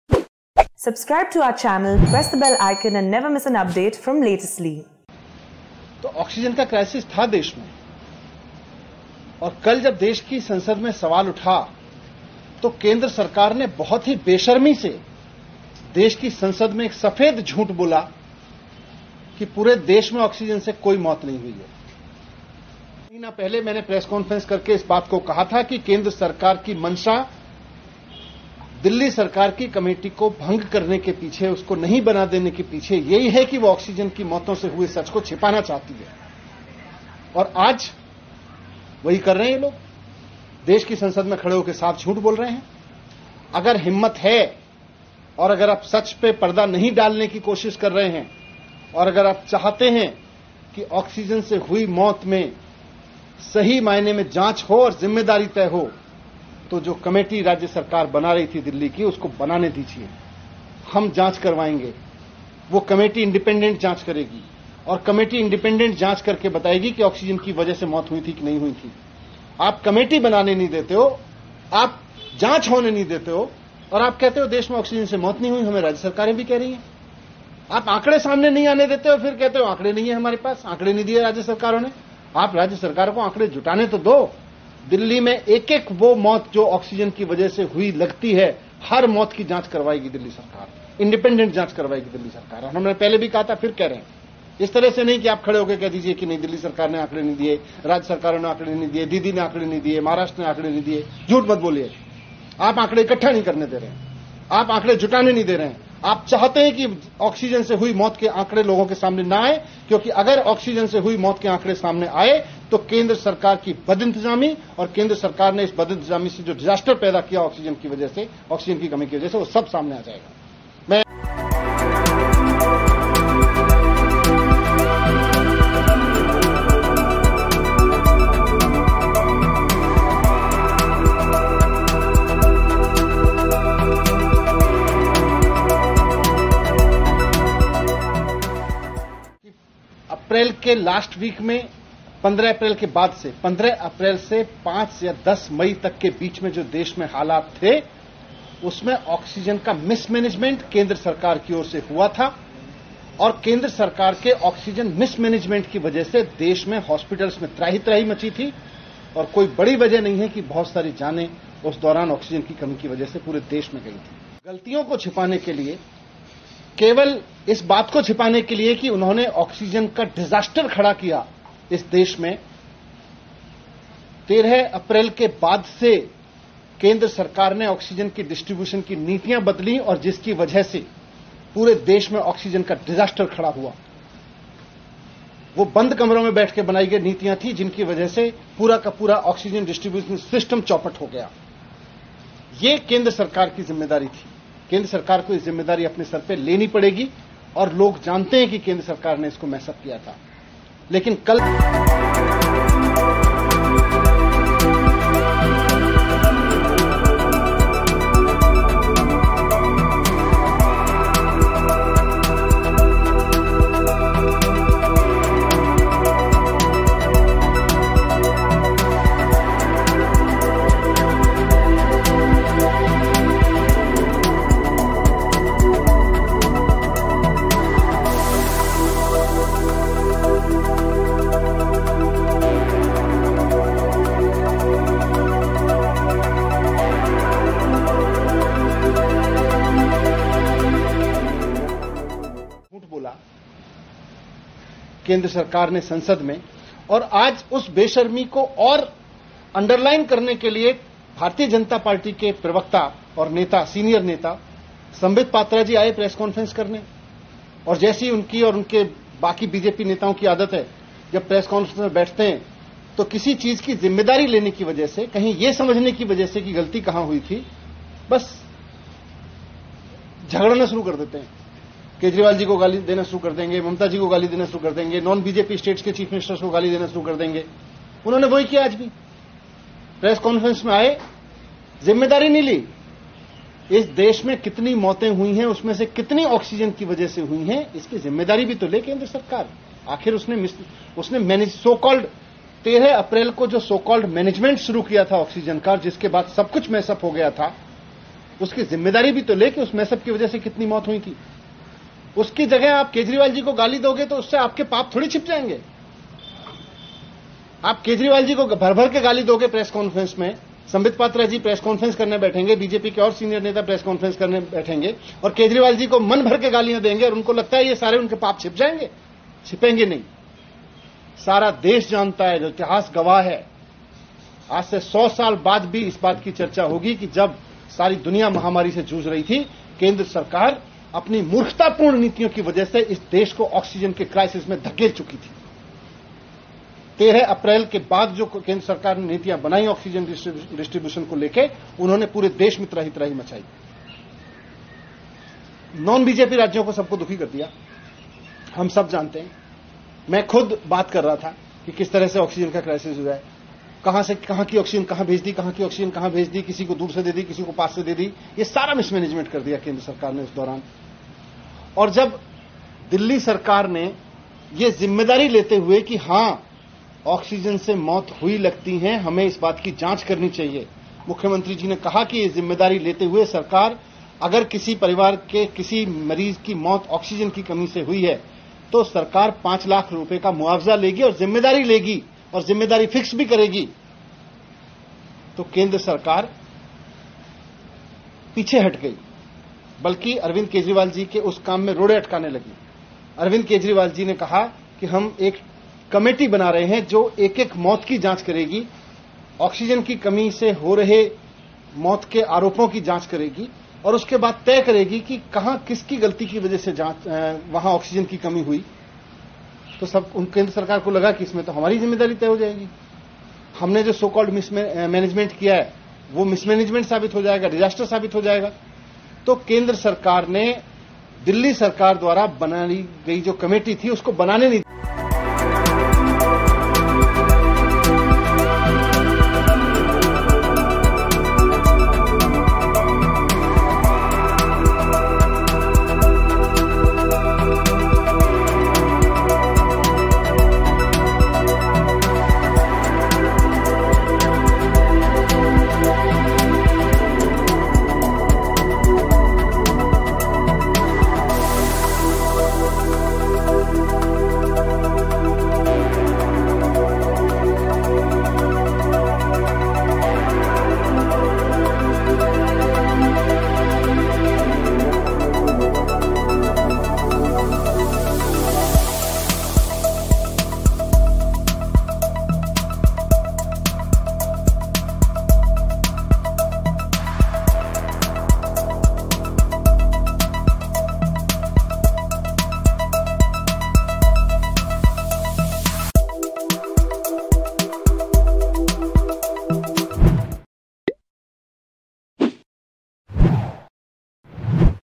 No Deaths Due To Oxygen-Shortage in Second COVID-19 Wave? Manish Sisodia Says Centre Blocking Audit
Upon being asked to furnish details of deaths due to Oxygen-scarcity during the second COVID-19 wave in India, the Modi Govt in Parliament chose to say that there were no such deaths. The reason for this answer being that states had not furnished the numbers to the Union government. However, Manish Sisodia, Delhi’s Deputy Chief Minister said that the Delhi government did not have any data regarding deaths due to the lack of oxygen as the Centre had not let it form a committee to look into the deaths of people, who allegedly died because of the lack of oxygen in hospitals between April and May.